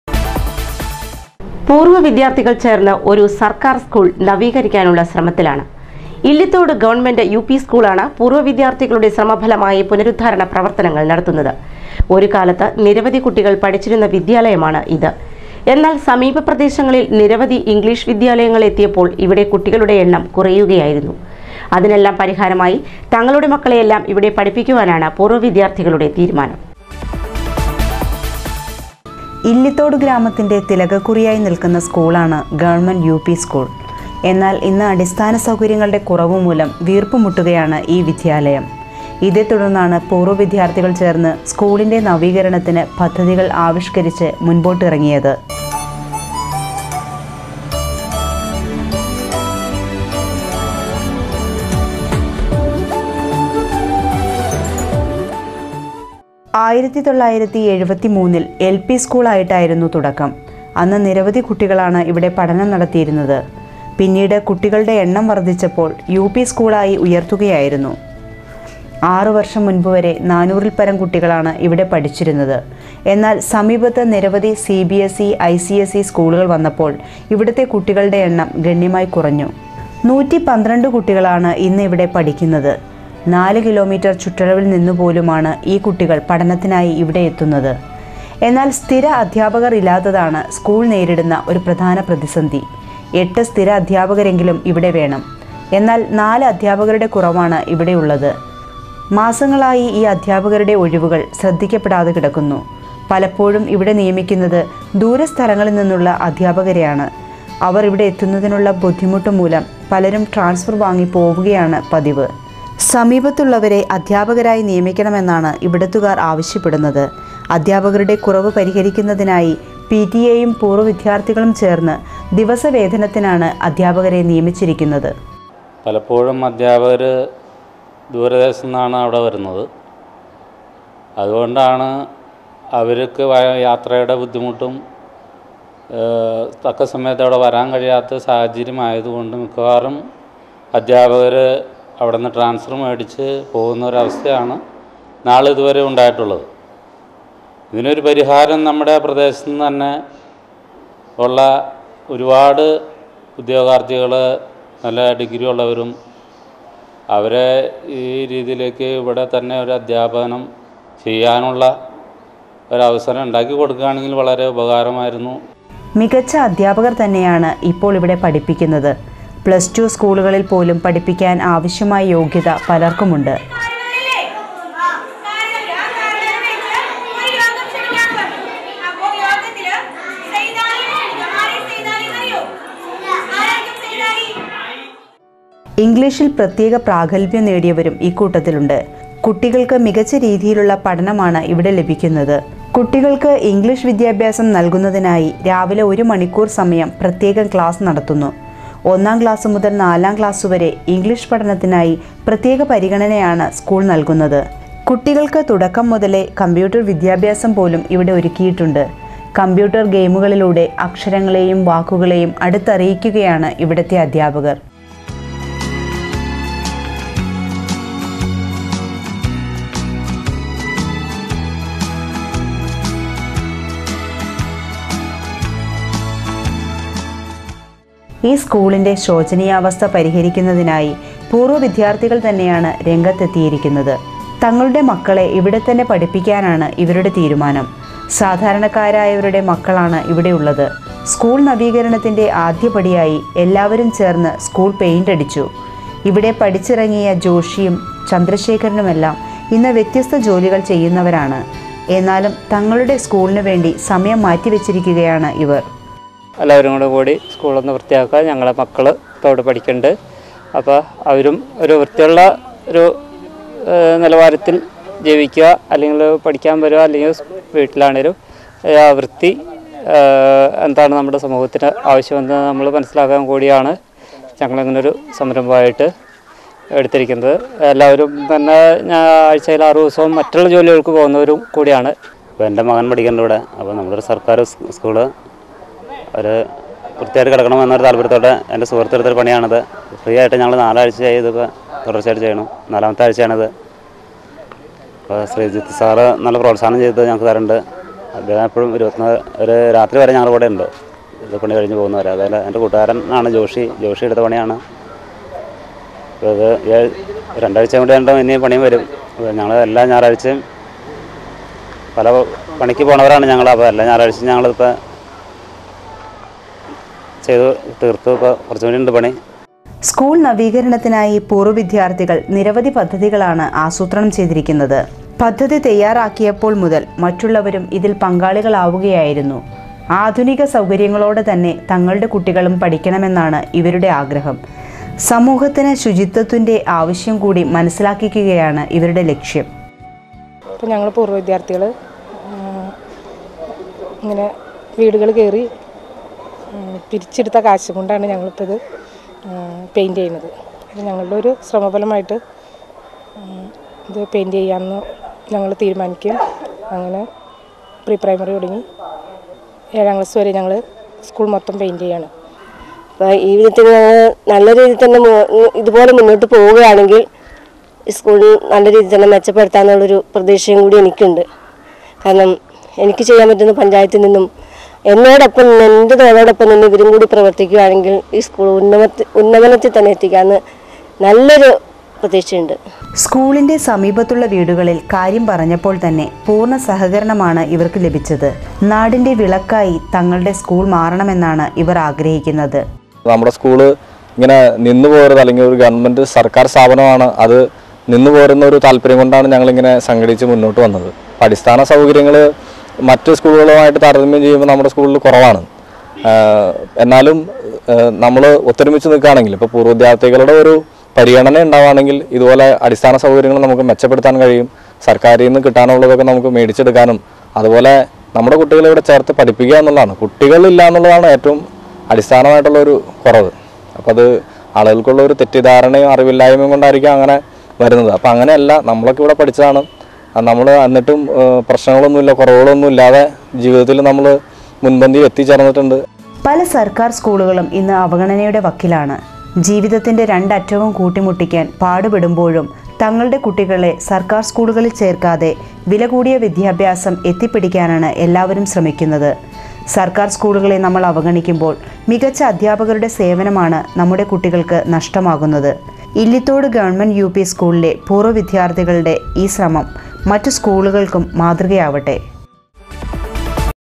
ILLITHODE UP SCHOOL
മലയാറ്റൂര്‍ ഇല്ലിത്തോട് യൂ.പി സ്ക്കൂള്‍ പൂര്‍വ്വവിദ്യാര്‍ത്ഥികളുടെ നേത്യത്വത്തില്‍ പുനരുദ്ധരിക്കുന്നു